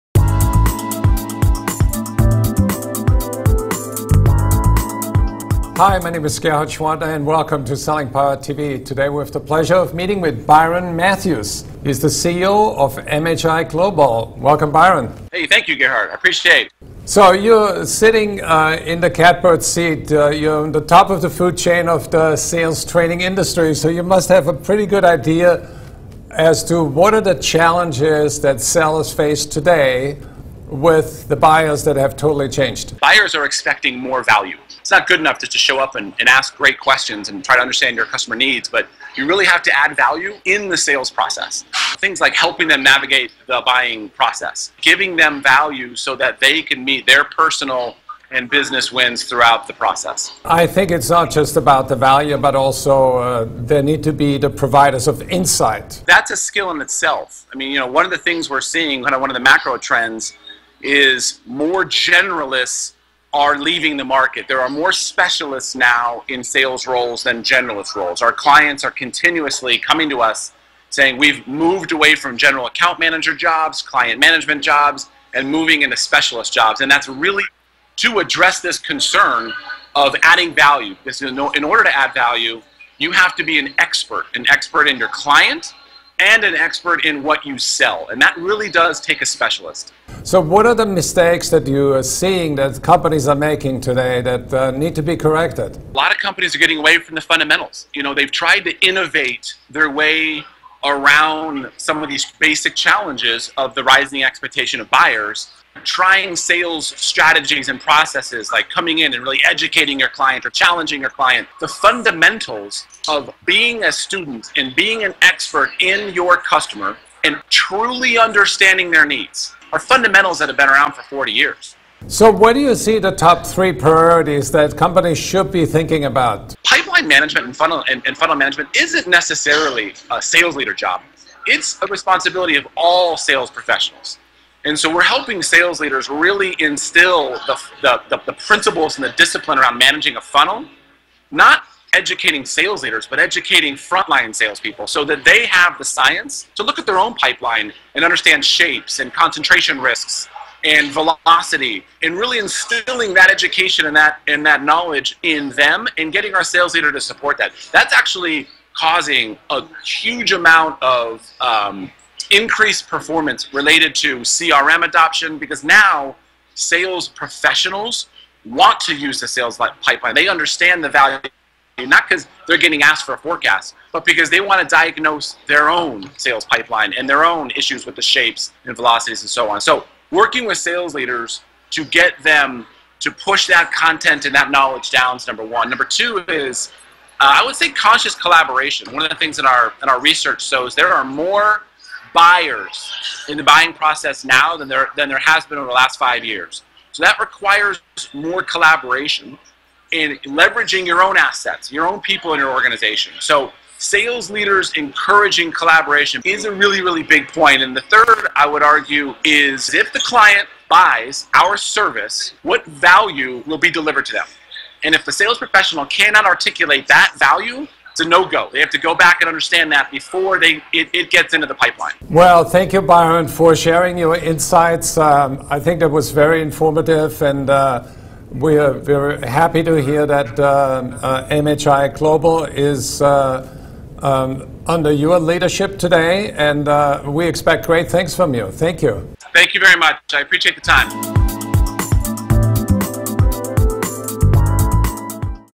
The greatest challenges for selling to today's buyers
Byron Matthews, CEO of MHI Global shares his how sales organizations need to change to stay relevant by creating more value for their buyers.